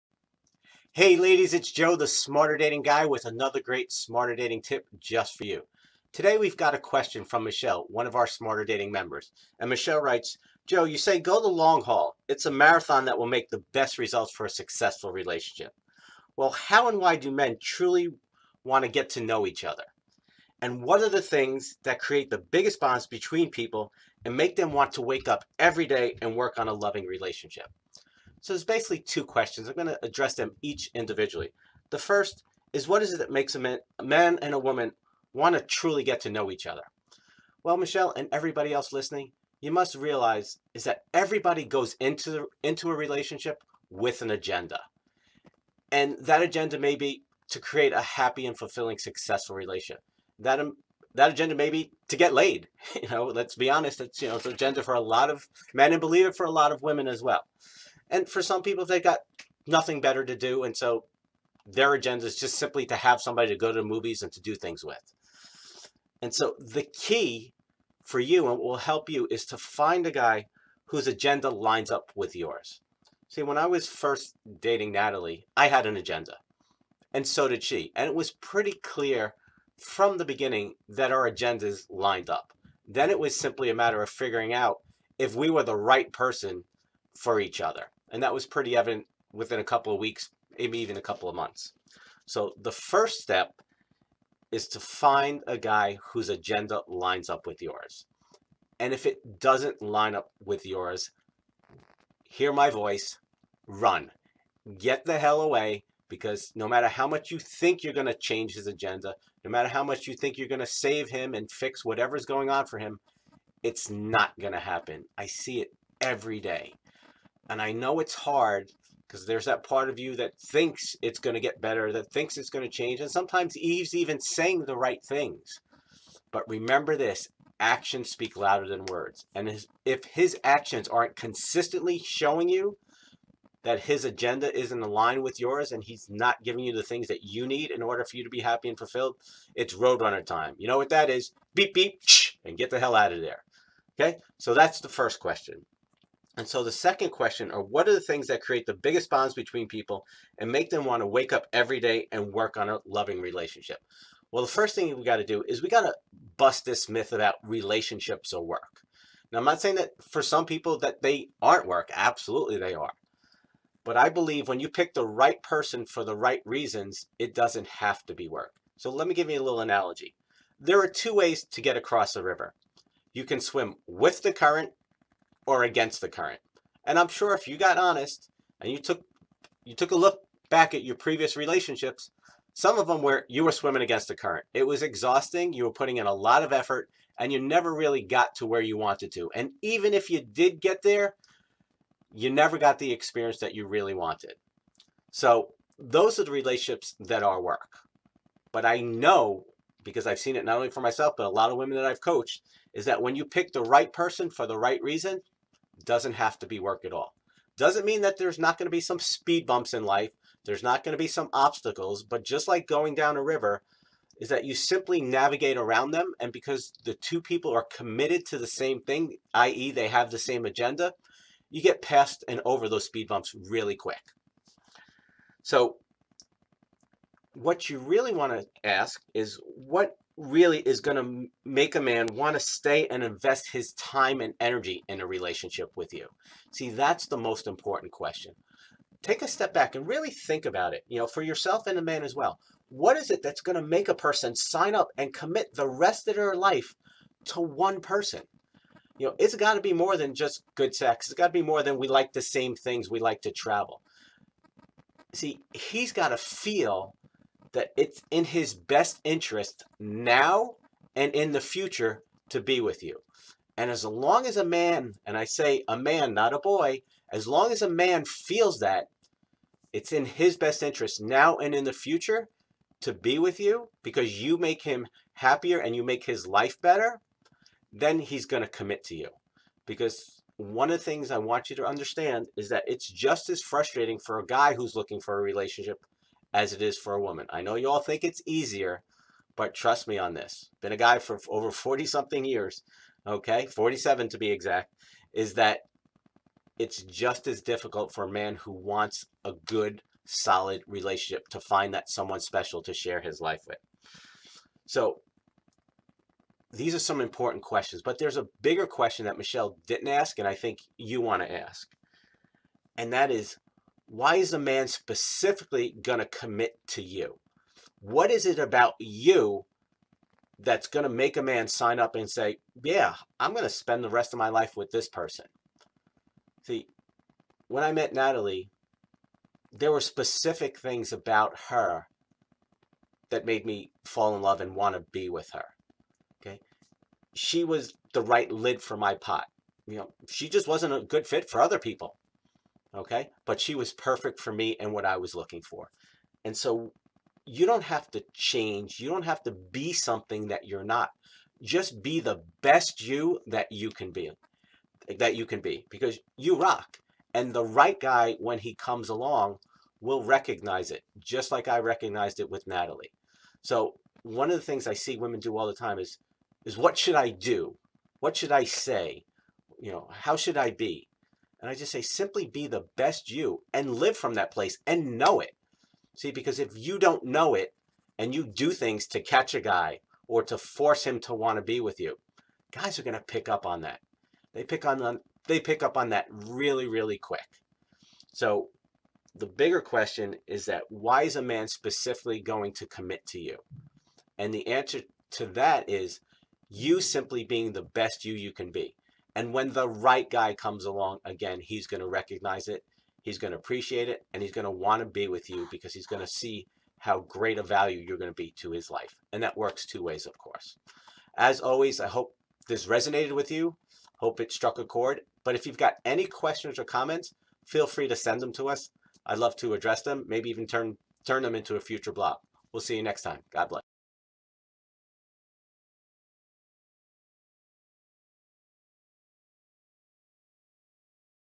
Insights Into The Male Mind - Why Men Commit
Joe Amoia, The Smarter Dating Guy, gives you a glimpse behind the curtain of the male mind and helps you to understand why some men willingly embrace commitment and why some guys run from it